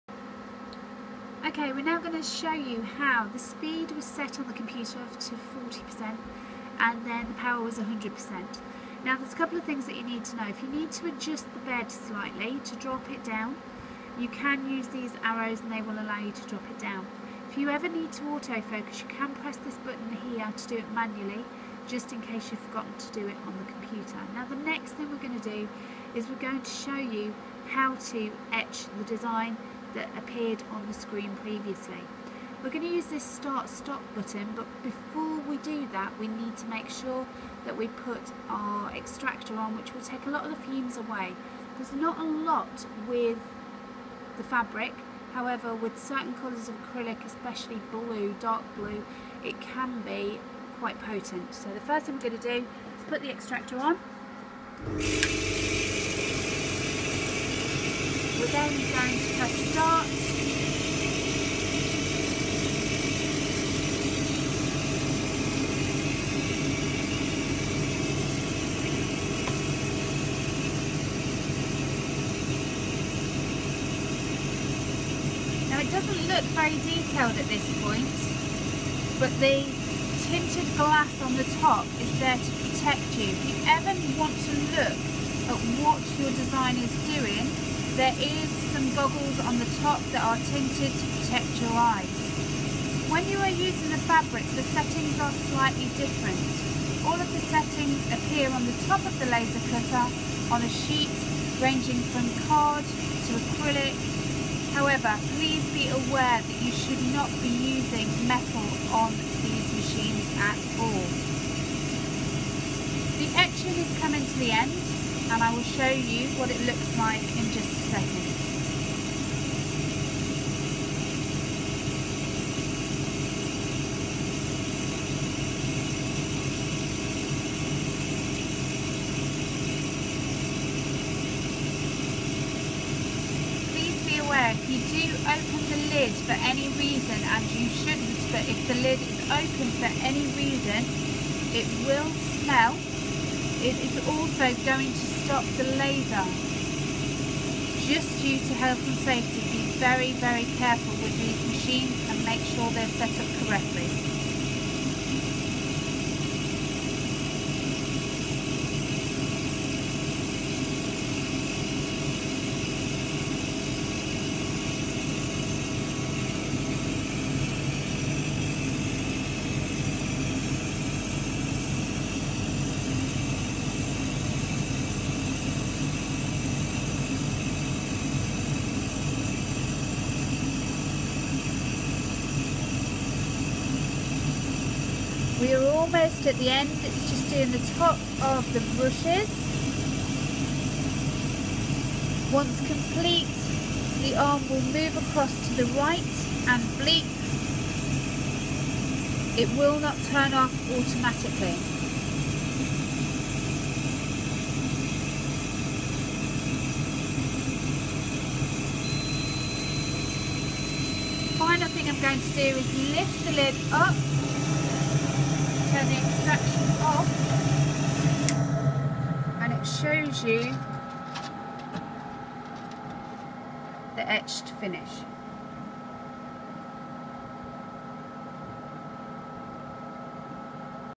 Setting the laser and etching the fabric
Using the appropriate settings the laser will etch fabric. This will give a reverse embossed effect and can be effective in small areas or patchwork pieces